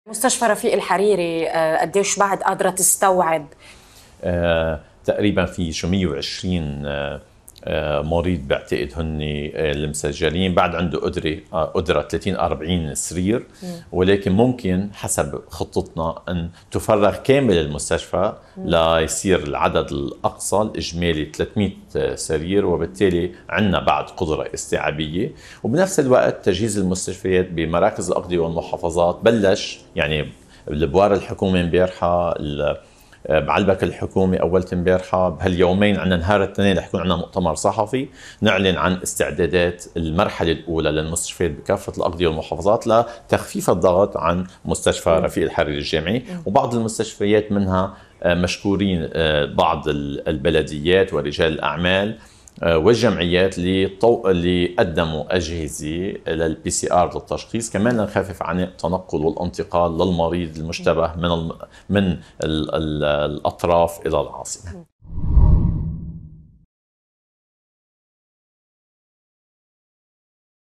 وزير الصحة اللبناني للغد: هناك اتجاه لتحويل مستشفى رفيق الحريري لمرضى الكورونا فقط
أكد وزير الصحة اللبناني حمد حسن أن هناك اتجاه لتحويل مستشفى رفيق الحريري لمرضى الكورونا فقط، مشيرا إلى أن لبنان يحاول أن يبقى ضمن دائرة الانتشار المحدود كي لا يتحول الفيروس إلى الانتشار المجتمعي، مشيرا خلال مقابلة خاصة مع قناة الغد حول جهود مكافحة انتشار فيروس كورونا في لبنان، أجرتها مراسلة الغد من بيروت ليلى خليل، أنه وبحسب تصنيف منظمة الصحة العالمية بتنا في اواخر المرحلة الثالثة، وأن انتشار الفيروس لا يزال محدود جغرافيا في لبنان.